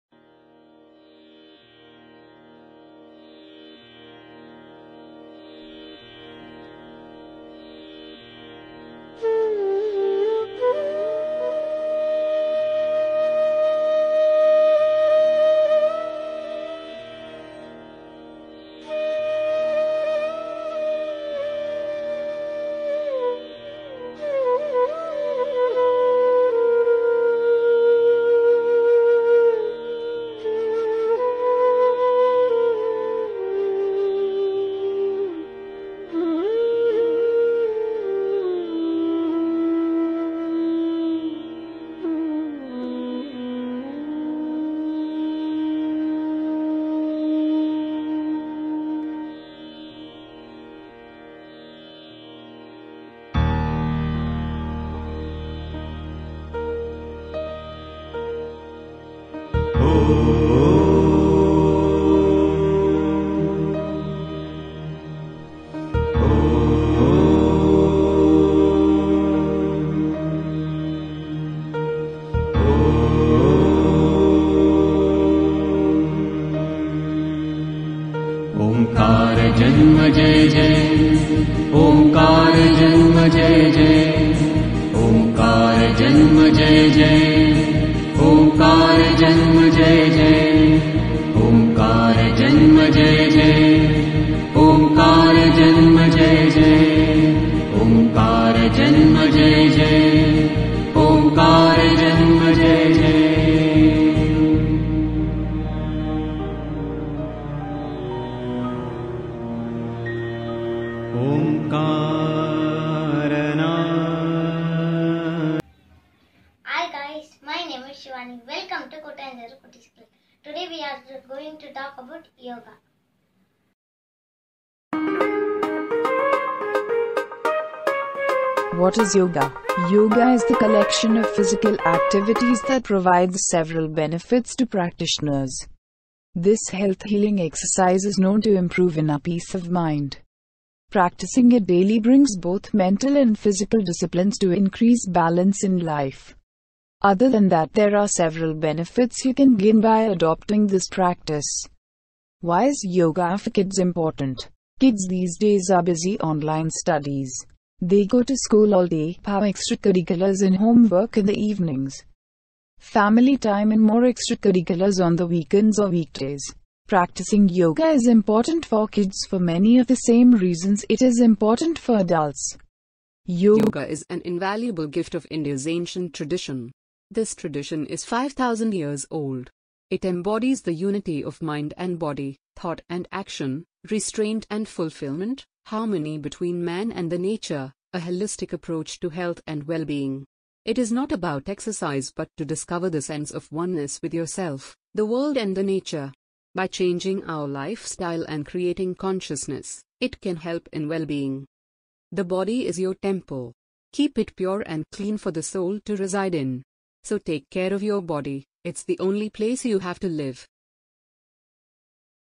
YOGA-யோகாசனம் -BY O.SHIVANI
Yoga is the collection of physical activities that provides several benefits to practitioners. This health healing exercise is known to improve inner peace of mind. Practicing it daily brings both mental and physical disciplines to increase balance in life. Other than that, there are several benefits you can gain by adopting this practice.
Why is yoga for kids important?
Kids these days are busy online studies. They go to school all day, have extracurriculars and homework in the evenings, family time and more extracurriculars On the weekends or week days  . . . Practicing yoga is important for kids for many of the same reasons it is important for adults. Yoga helps kids to relax, relieve stress and anxiety, sleep better, improve emotional regulation, increase empathy, and improve mood and affect.
Why is Yoga Good for Kids?
Besides the simplicity of practice, the benefits of yoga for children is extensive.  From young children to teenagers, inactive kids to serious athletes, yoga can help: Increase in concentration, Lower stress, Improve body control and awareness Reduce the risk of injury and that’s just a small sampling of the benefits.  Kids can practice yoga on their own or it can be used as a great social tool or family workout.  Because yoga is non-competitive, it can be done in groups of individuals with very different athletic abilities.

 I just want to remember our national father Mahatma Gandhi's life philosophy. He said, “It is health that is real wealth and not pieces of gold and silver.”

As we know, health is the essential thing in life, and healthy living is the way to progress. The art of practicing yoga helps in controlling an individual's mind, body, and soul. It brings together physical and mental disciplines to achieve a peaceful body and mind; it helps manage stress and anxiety and keeps you relaxing. It also helps in increasing flexibility, muscle strength, and body tone it improves respiration, energy, and vitality. Practicing yoga might seem like just stretching, but it can do much more for your body from the way you feel, look, and move. Yoga asanas build strength, flexibility, and confidence. Regular practice of yoga can help lose weight, relieve stress, improve immunity Improves immunity, Fights against the thyroid, Increases determination level, Brings positive energy, Increases concentration level, Burns unwanted calories, Purifies digestive disorder, Improves kidney functions and maintain a healthier lifestyle.

 "Yoga is an invaluable gift of India's ancient tradition. This tradition is 5000 years old. It embodies the unity of mind and body; thought and action; restraint and fulfillment; harmony between man and the nature; a holistic approach to health and well-being. It is not about exercise but to discover the sense of oneness with yourself, the world, and nature. By changing our lifestyle and creating consciousness, it can help in well-being.
“The body is your temple”. Keep it pure and clean for the soul to reside in. So Take care of your body, it’s the only place you have to live.